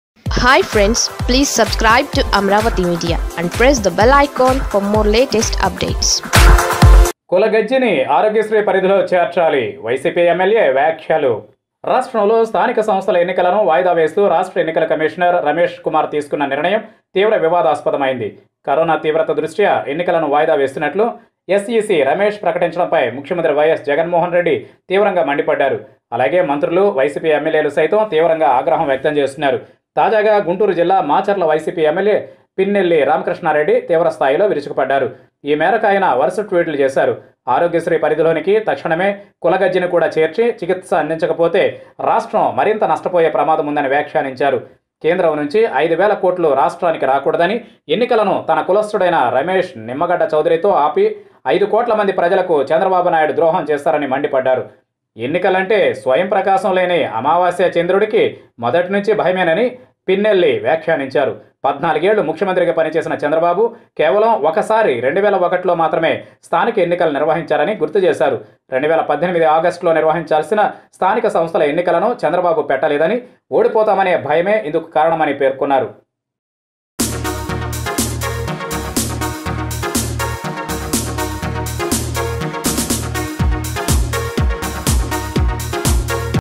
కులగజ్జిని ఆరోగ్యశ్రీలో చేర్చాలాట..పిన్నెల్లి సెటైర్లు| YCP MLA Pinnelli Satires on Chandrababu
YSRCP MLA Pinnelli Ramakrishna Reddy Criticized TDP Chief Chandrababu Naidu and SEC Nimmagadda Ramesh Kumar over Postponement of Local Body Elections.